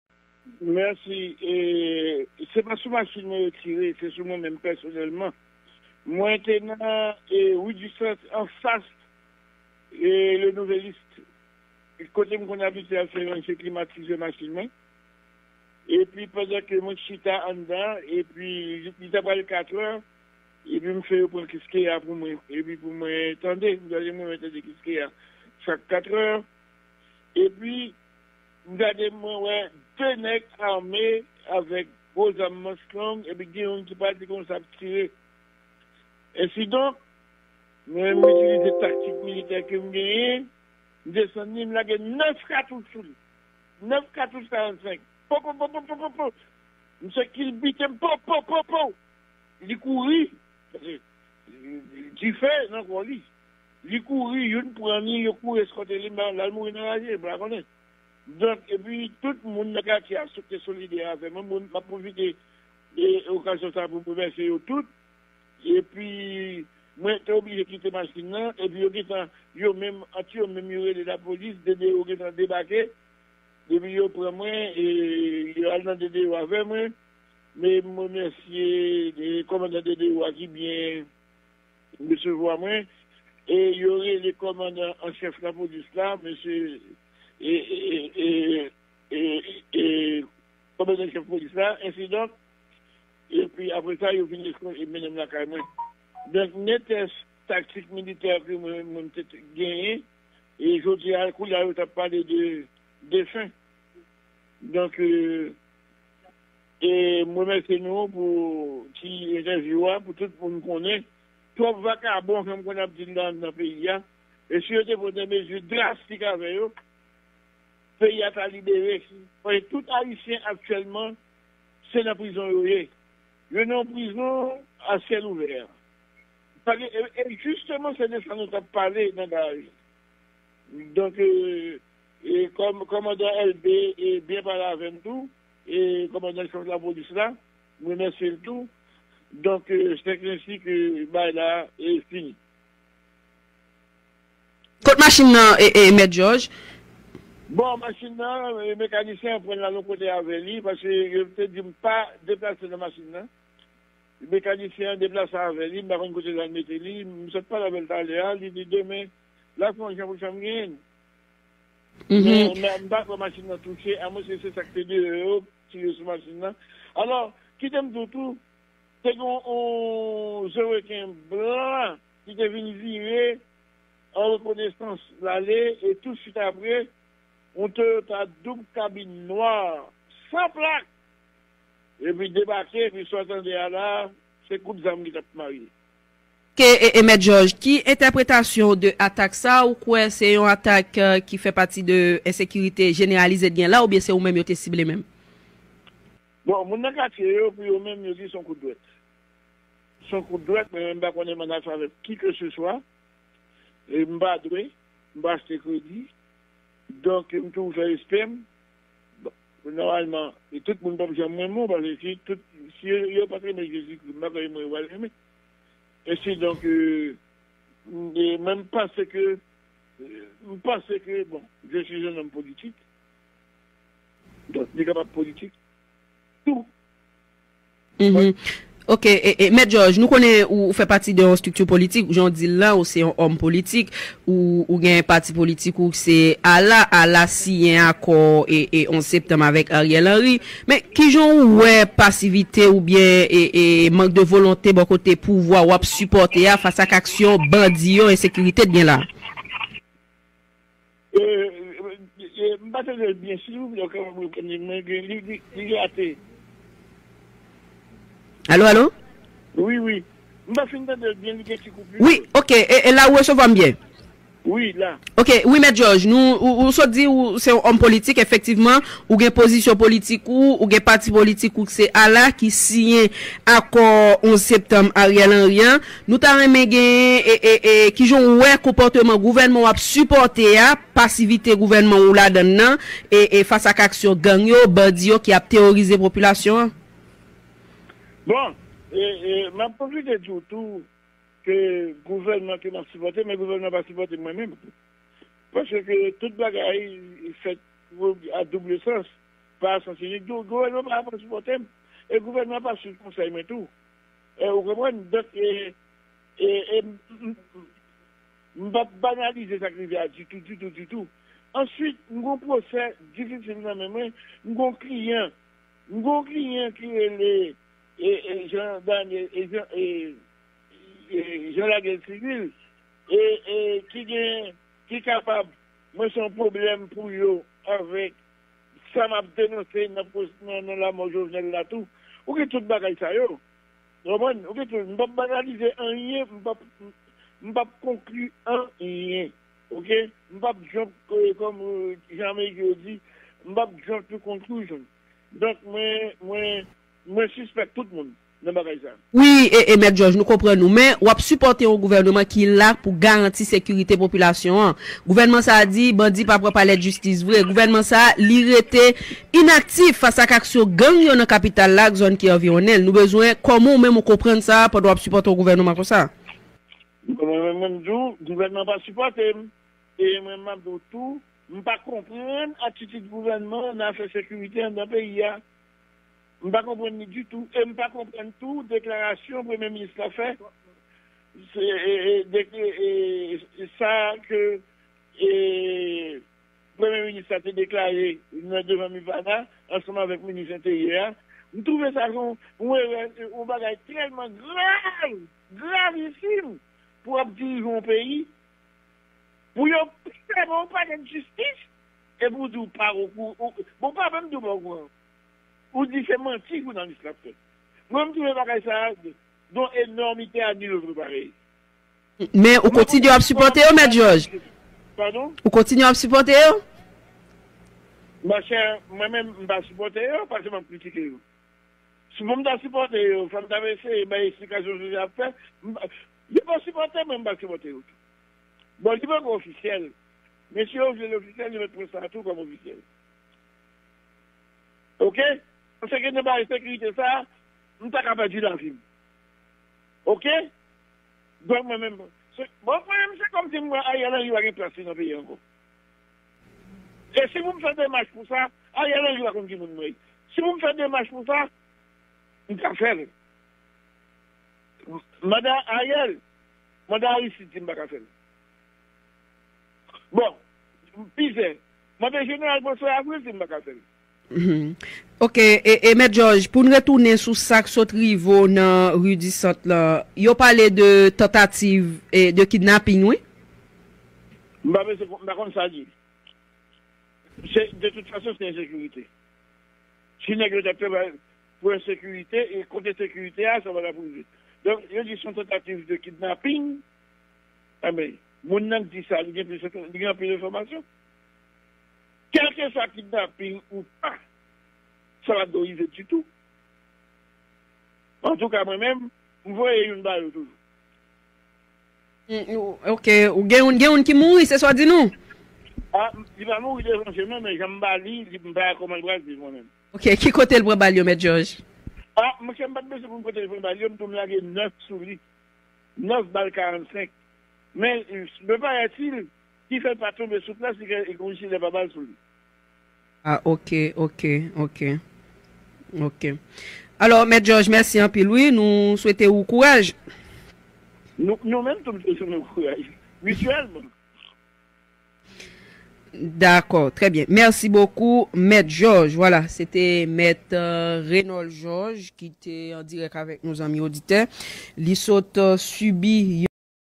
Mèt Georges manke nan kouri!Li sibi yon tantativ anlevman se grasa teknik militèl kifè yopa ale avèl
Mèt Reynold Georges manke nan kouri! Li sibi yon tantativ anlevman se grasa teknik militè l ki fè yo pa ale avèl

Jouna9#HaitianNews# LiveJounal#LiveNews